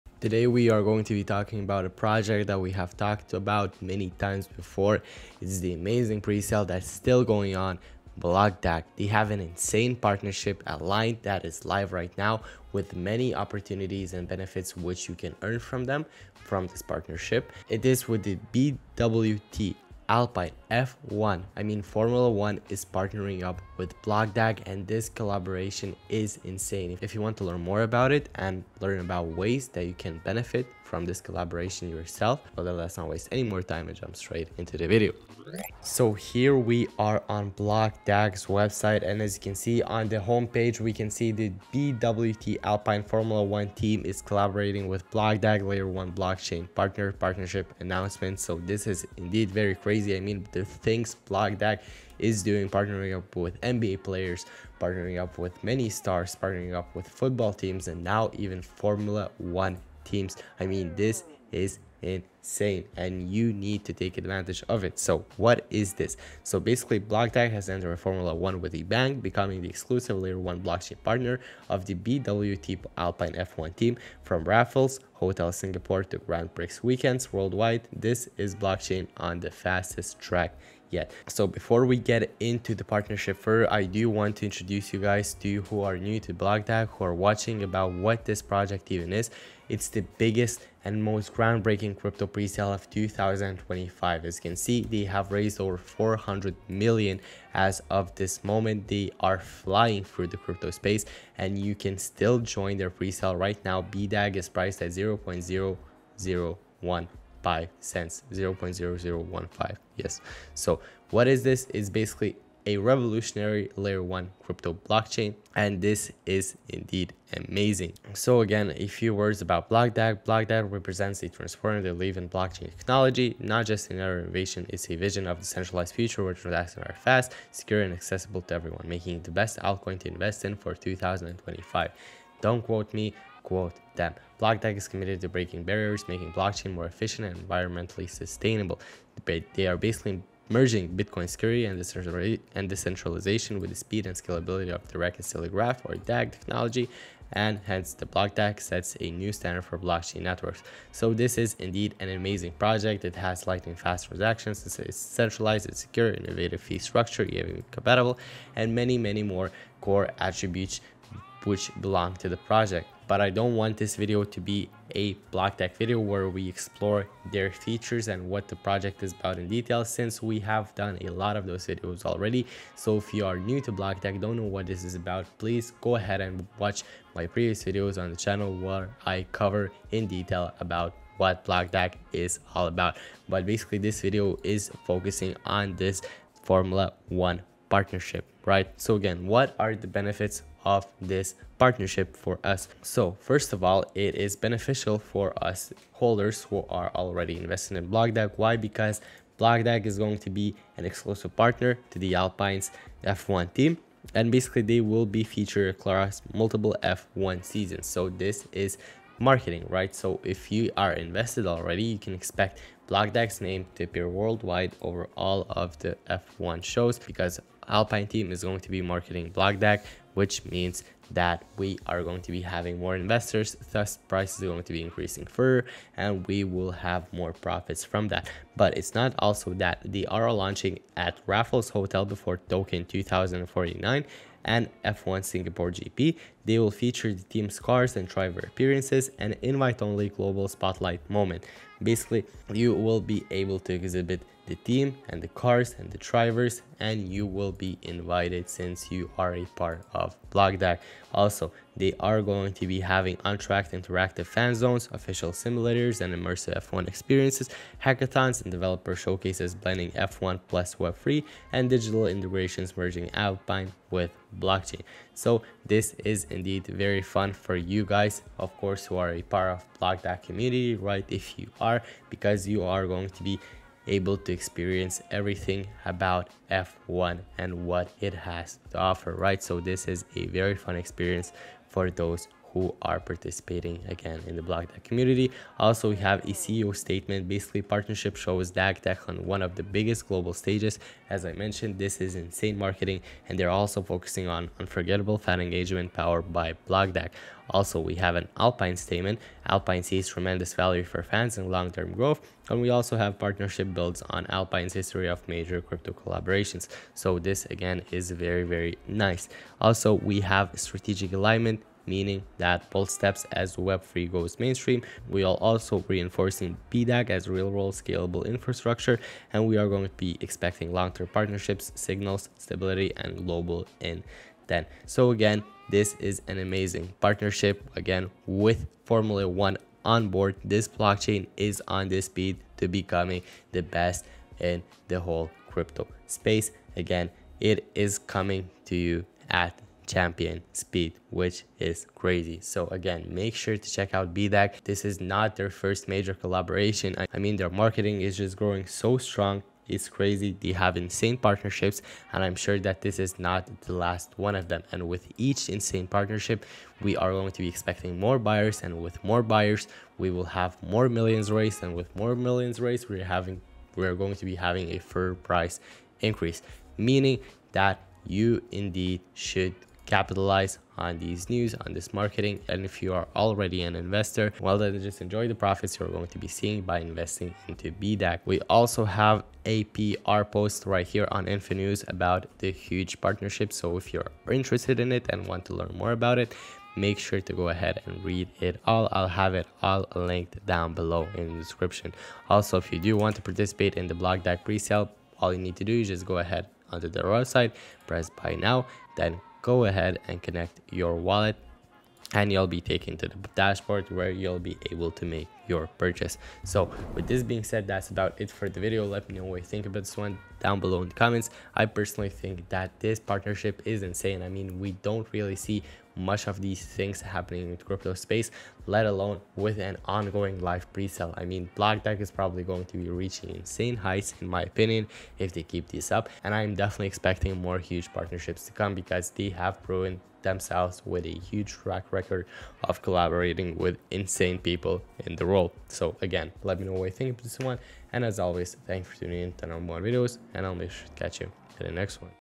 🏎️ BlockDAG ($BDAG) | The Fastest Blockchain in the World 🌐 Built for Speed & Innovation
🏎️ Formula 1 speed. Blockchain innovation. That’s BlockDAG ($BDAG).
The world’s fastest-growing blockchain project, now the Exclusive Layer 1 Partner of the BWT Alpine F1® Team — taking blockchain tech to championship speed.

⚡ Why BlockDAG Is Built Different
- DAG-based architecture for ultra-fast, high-throughput transactions.
- Zero bottlenecks — near-instant confirmations and scalable performance.
- Low fees and sustainable design for real-world use cases.
- Multi-year F1® partnership driving global visibility and credibility.
- Fan simulators, Web3 hackathons, and F1® activations around the world.

💎 The Future of Blockchain Speed
BlockDAG’s technology outpaces traditional chains by merging **decentralized architecture** with **real-world integrations**.
It’s built for both speed and adoption — the ultimate fusion of blockchain power and F1® innovation.

🌍 Global Expansion
From Singapore to Monaco and beyond, BlockDAG is everywhere Formula 1® goes — connecting blockchain with mainstream audiences through world-class racing events.

💬 CEO Antony Turner:
“Our mission is simple — to bring blockchain performance up to Formula 1 standards.”

🏁 Formula 1 runs on speed — and now, so does blockchain.